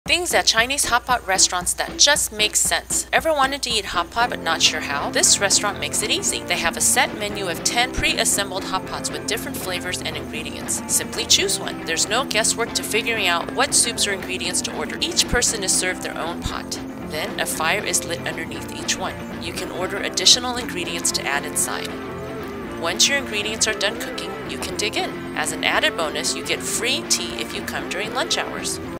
THINGS AT CHINESE HOT POT RESTAURANTS THAT JUST MAKE SENSE
Boiling Point

Things at Chinese Hot Pot restaurants that just make sense.  This restaurant makes it easy and simple to enjoy delicious hot pot!  They have a pre-set menu of 10 different choices of hot pots.  Simply pick your pot and enjoy!